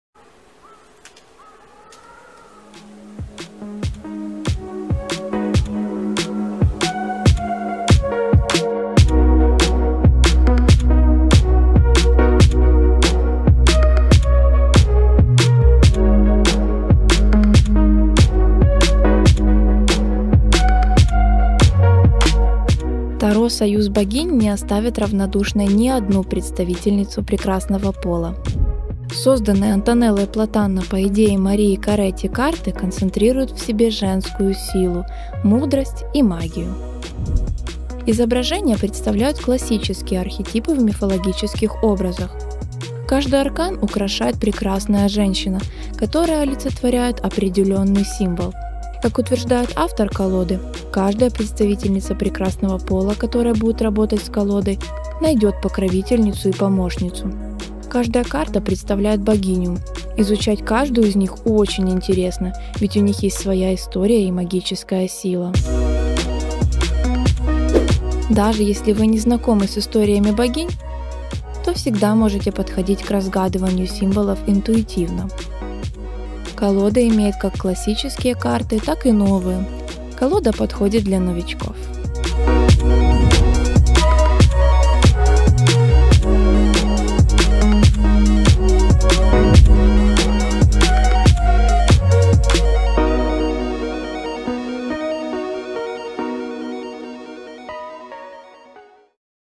Таро Союз Богинь
Карты таро Союз БогиньТаро Союз Богинь можно назвать женским Таро. И не ошибиться. Здесь главными героинями являются богини. Со всех континентов. Каждая из них имеет свою историю. Каждая является не только богиней, но и… Женщиной. Матерью, дочерью, бабушкой, женой, возлюбленной… В их образах искусно сочетаются Сила и Мудрость, Красота и Волшебство. Каждая из богинь уникальна, поэтому любая из женщин сможет найти себе среди них покровительницу. Всегда считалось, что женская сила и магия соседствуют друг с другом. Это Таро является прекрасный иллюстрацией этих слов. Волшебство домашнего очага, материнства, любви и женственности прекрасно сочетаются с могущественными Силами, которыми обладают богини. Злые колдуньи, добрые волшебницы, заботливые ведуньи, коварные обольстительницы, таинственные предсказательницы, великие воительницы – в Таро Союз Богинь они прекрасно соседствуют друг с другом. Прекрасна эта колода и тем, что во время работы с ним Вы сможете познакомиться с культурами разных стран и народов.